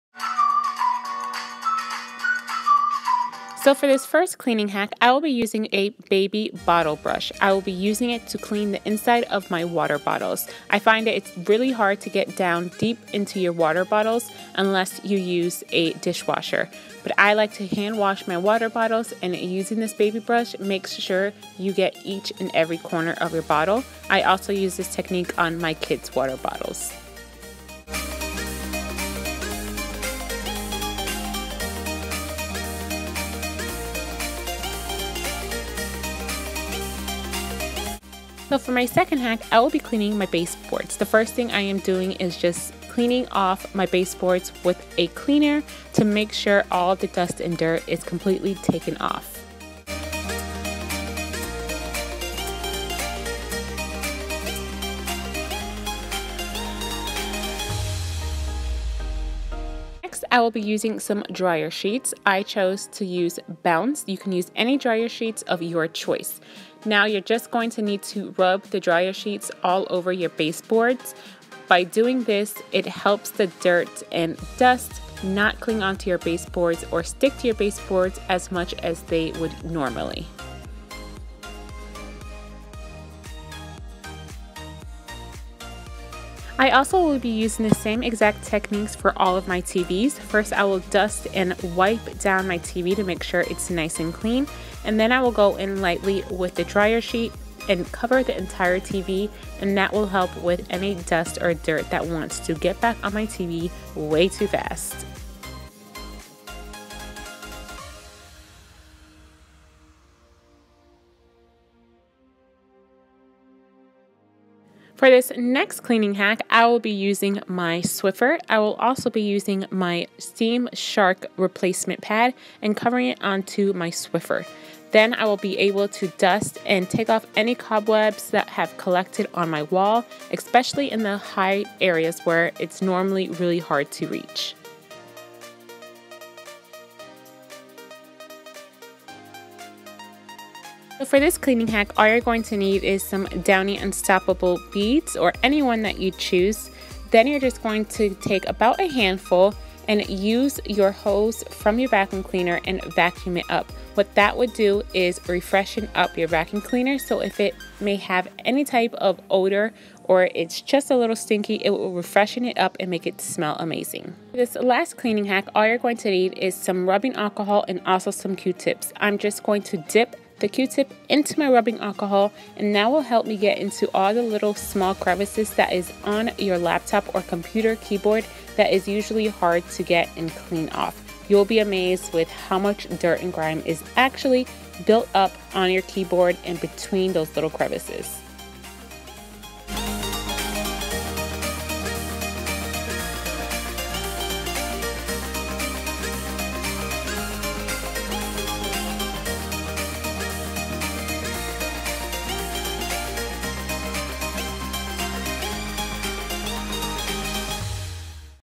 Cleaning Hacks
Hey Guys! I decided to share with you 5 cleaning hacks, that I like to use. I love getting new ideas to cleaning my home, so I hope I may have given you some ideas to try out on your own.
If you missed my past cleaning hack video, the link will be down below if you would like to check it out and get some more ideas!

jillianstarling

Ello Water Bottle :

Filming Equipment :

iPhone 6+

Tripod Stand